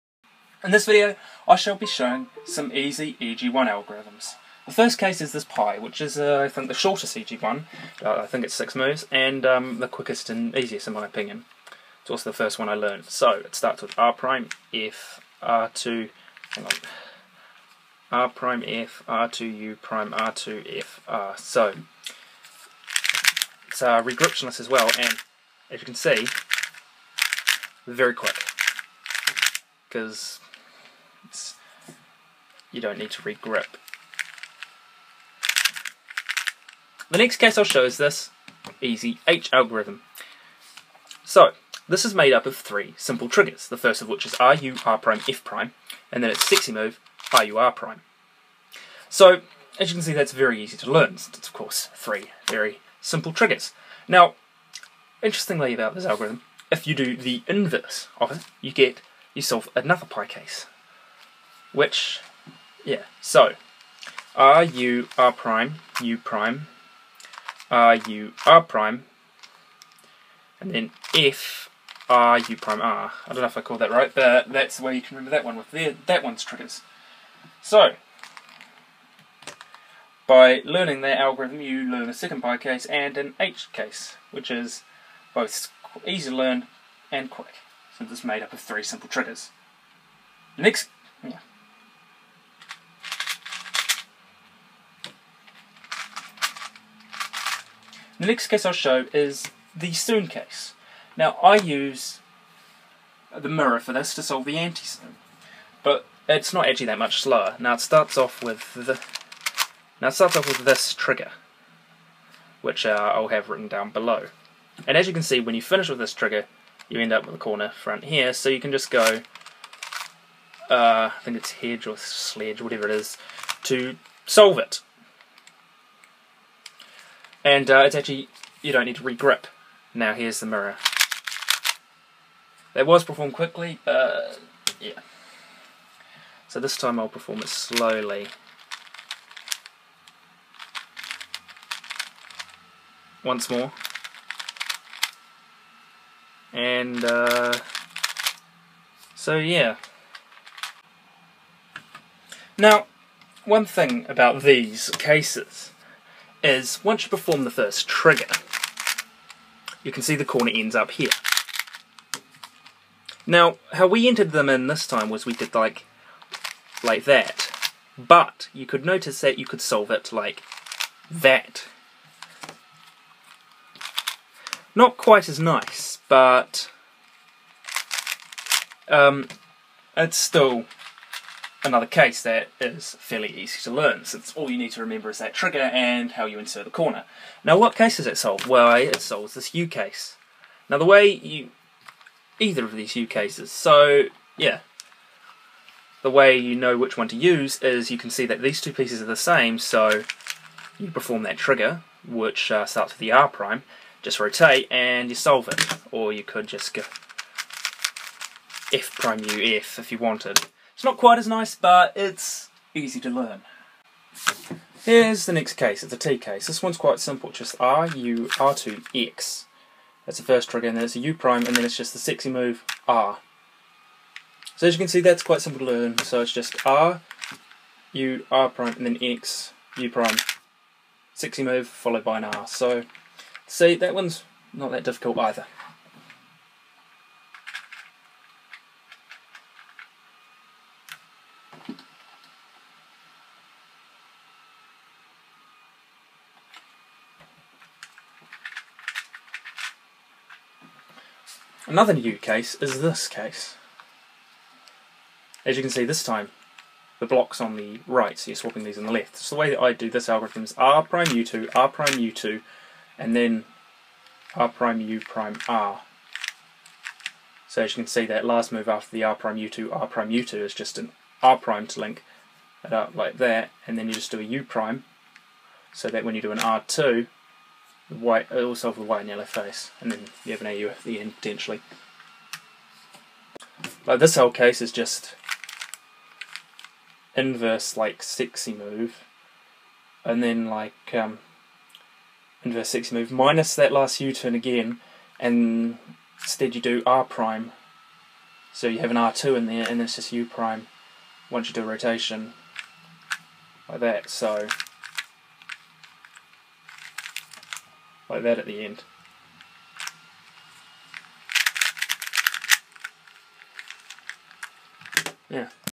Easy EG-1 Algs
Fast and easy to learn EG-1 algs. Hope this helps you learn them.